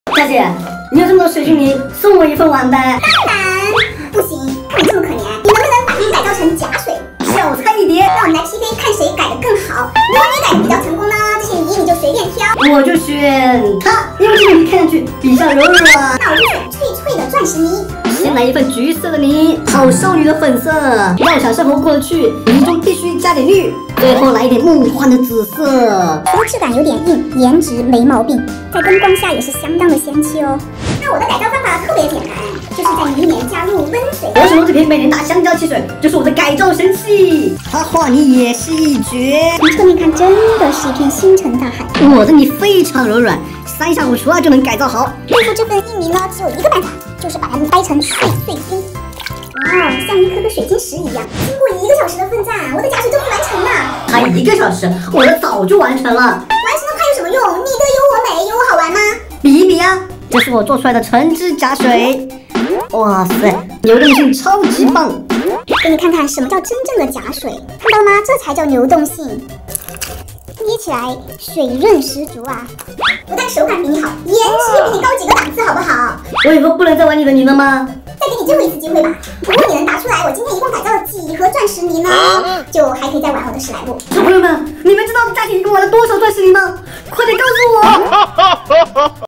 【架子手作】工廠脆泥大拯救，清水PK香蕉汽水洗無硼砂泥，洗出一片星辰大海
架子姐姐和小陽PK改造假水，小陽在水晶泥裏加入了美年達香蕉汽水，架子姐姐加入了溫水，架子姐姐的泥真的超級好看啊~小陽的……你們覺得誰的假水改造的更加成功呢？小可愛們幫幫小陽，架子姐姐一共改造了幾盒鑽石泥呢？
感謝小可愛們的訂閲： 本频道的内容都经过专业审核，确保不会包含任何可能对儿童产生负面影响的元素。